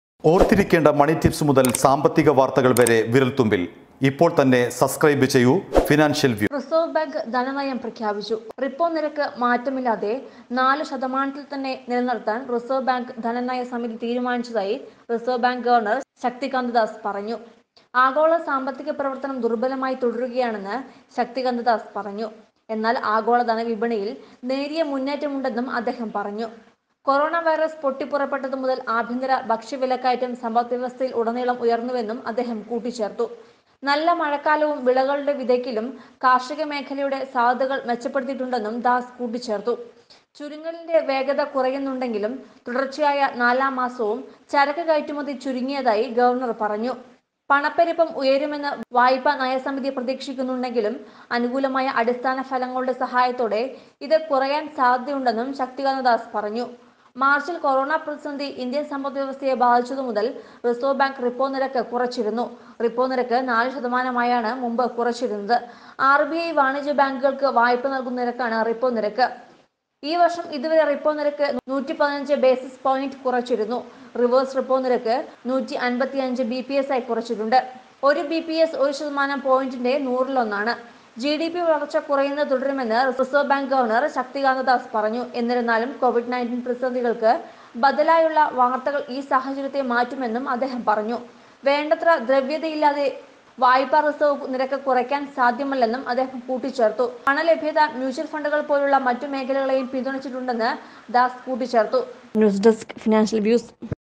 റിപ്പോ നിരക്കില്‍ മാറ്റമില്ലെന്ന് റിസര്‍വ് ബാങ്ക് ധനനയ പ്രഖ്യാപനം l Reserve Bank of India
റിസര്‍വ് ബാങ്ക് ധനനയം പ്രഖ്യാപിച്ചു. റിപ്പോ നിരക്ക് മാറ്റമില്ലാതെ, 4 ശതമാനത്തില്‍ തന്നെ നിലനിര്‍ത്താന്‍ റിസര്‍വ് ബാങ്ക് ധനനയ സമിതി തീരുമാനിച്ചതായി റിസര്‍വ് ബാങ്ക് ഗവര്‍ണര്‍ ശക്തികാന്ത ദാസ് പറഞ്ഞു. ആഗോള സാമ്പത്തിക പ്രവര്‍ത്തനം ദുര്‍ബലമായി തുടരുകയാണെന്ന് ശക്തികാന്ത ദാസ് പറഞ്ഞു. എന്നാല്‍ ആഗോള ധനവിപണിയില്‍ നേരിയ മുന്നേറ്റമുണ്ടെന്നും അദ്ദേഹം പറഞ്ഞു. കൊറോണ വൈറസ് പൊട്ടിപ്പുറപ്പെട്ടതു മുതല്‍ ആഭ്യന്തര ഭക്ഷ്യവിലക്കയറ്റം സമ്പദ്വ്യവസ്ഥയിലുടനീളം ഉയര്‍ന്നുവെന്നും അദ്ദേഹം കൂട്ടിച്ചേര്‍ത്തു.